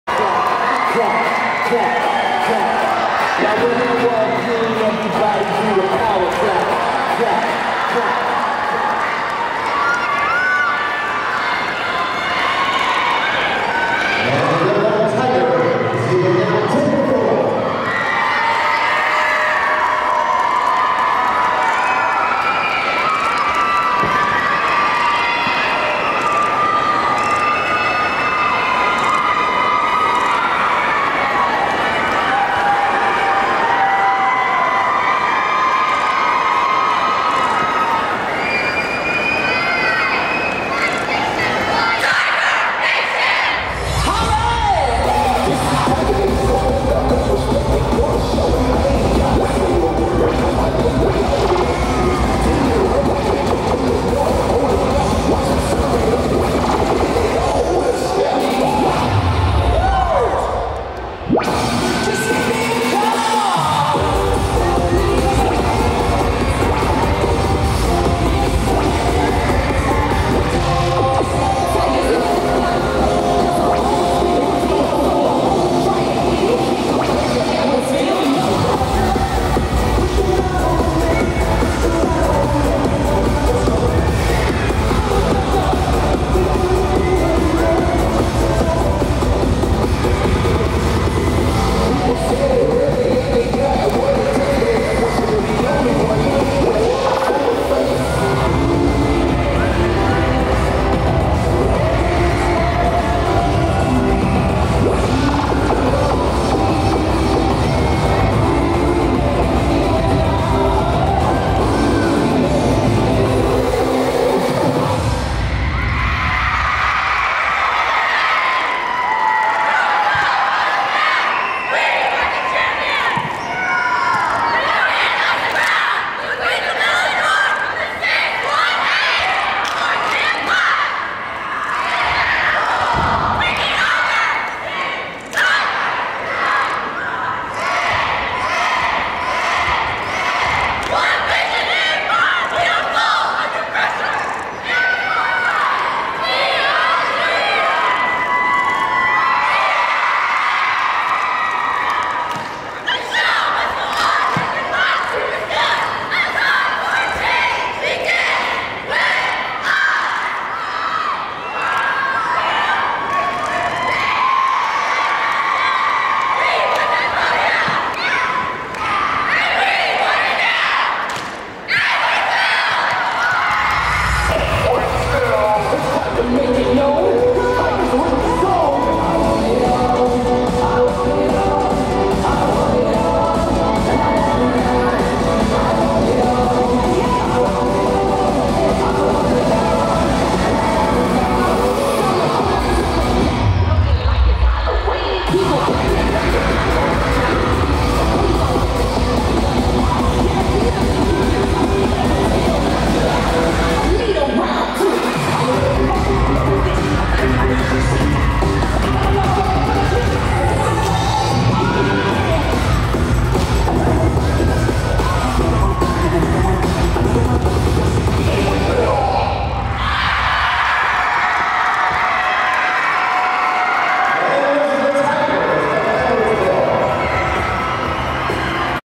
Edwardsville IHSA State Cheer Competition February 2020 Prelim Round
The girls competed at the IHSA Cheer Competition in Bloomington IL at The Grossinger Center. They placed 8th in the state. This video is the preliminary round.